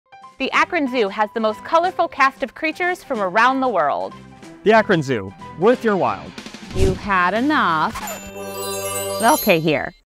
Akron Zoo - Worth Your Wild
The Akron Zoo is home to more than 2,000 animals from around the world, all with unique personalities that you have to see to believe!